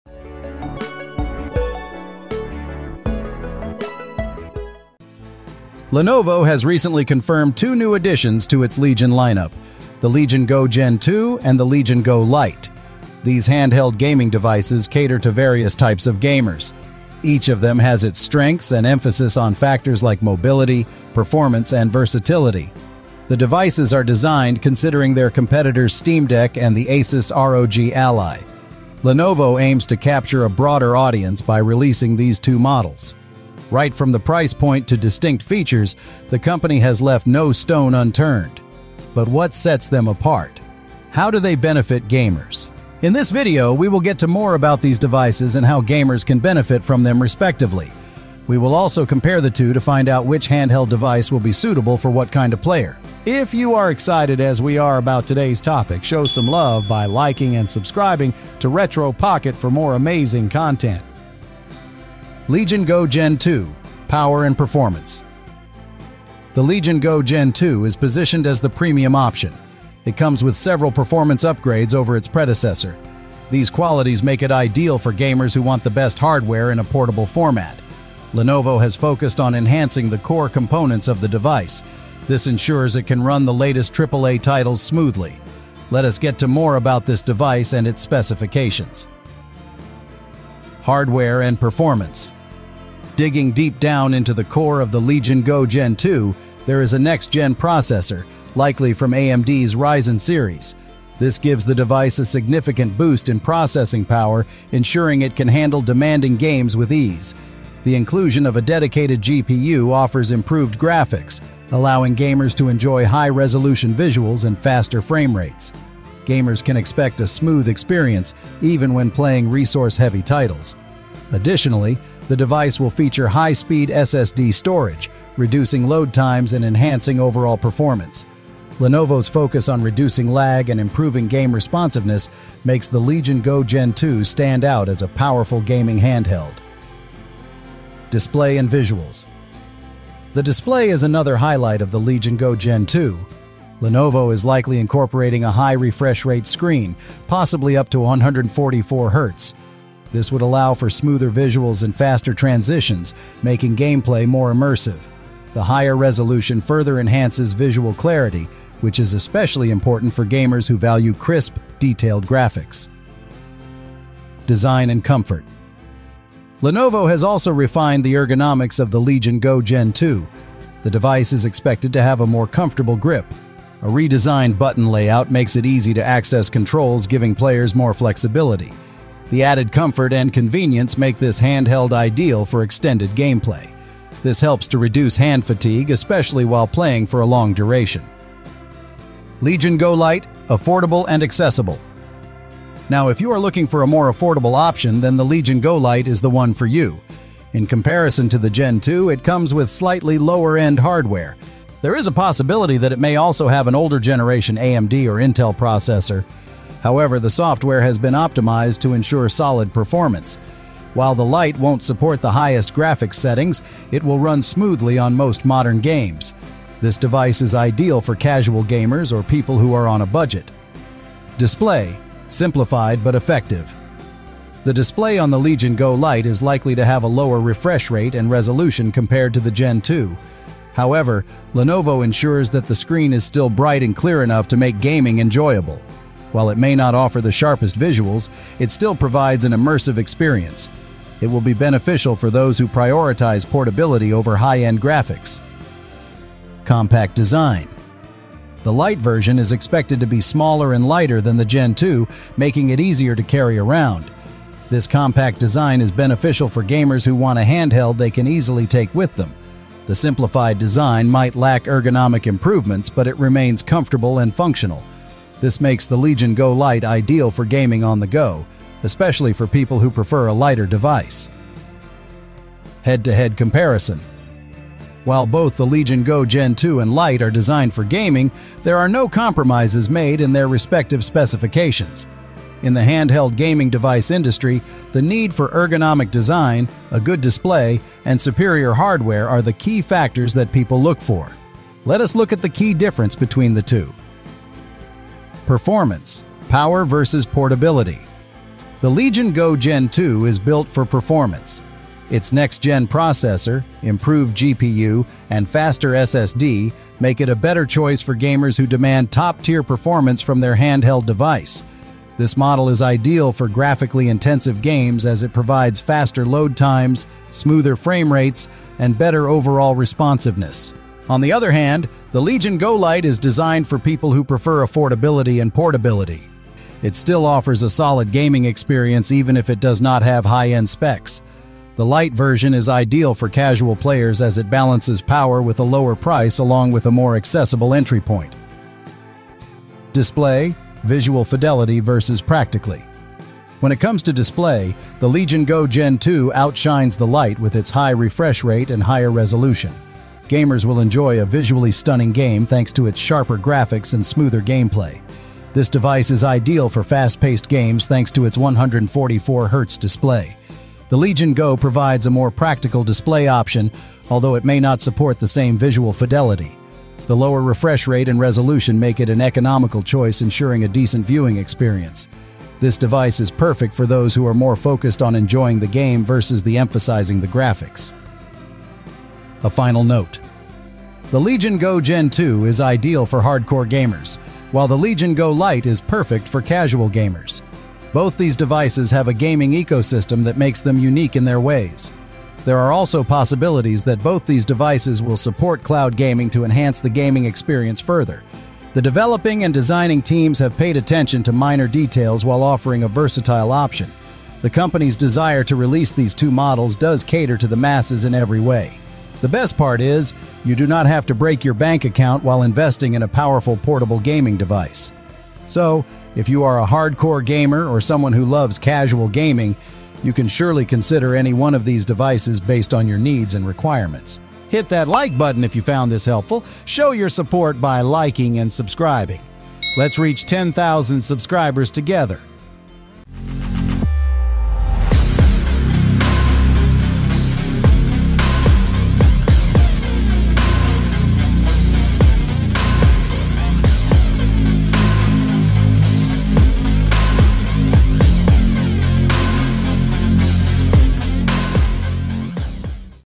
Lenovo's Legion Go Gen 2 and Legion Go Lite: A New Era of Handheld Gaming
Lenovo has officially confirmed the upcoming release of the Legion Go Gen 2 and Legion Go Lite handheld gaming consoles. This video dives deep into the details, exploring the potential features, specifications, and design elements of these exciting new devices. We'll discuss the improvements over the original Legion Go, including potential upgrades to the processor, display, and battery life. Join us as we speculate on the future of Lenovo's handheld gaming lineup and the exciting possibilities that await gamers.

0:00 Intro
1:01 Legion Go Gen Two: Power and Performance
1:29 Hardware and Performance
2:17 Display and Visuals
2:43 Design and Comfort
3:08 Legion Go Lite: Affordable and Accessible
3:41 Display: Simplified but Effective
4:07 Compact Design
4:33 Head-to-Head Comparison
4:55 Performance: Power vs. Portability
5:40 Display: Visual Fidelity vs. Practically
6:19 A final note